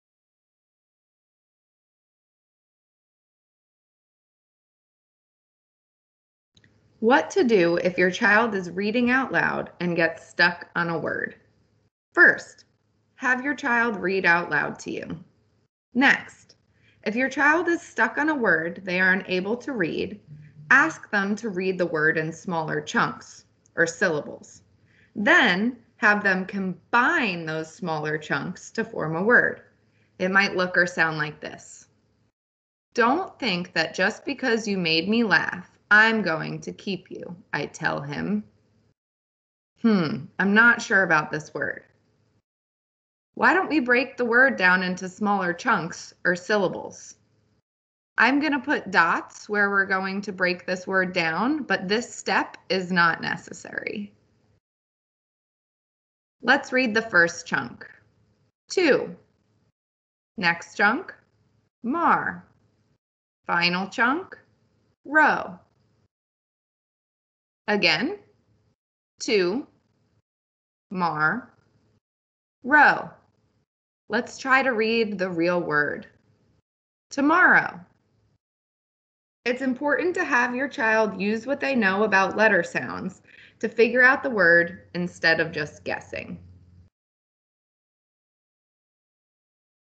Breaking a word down into syllables
1. Have your child read out loud
2. If your child gets stuck on a word, encourage them to break the word down into smaller chunks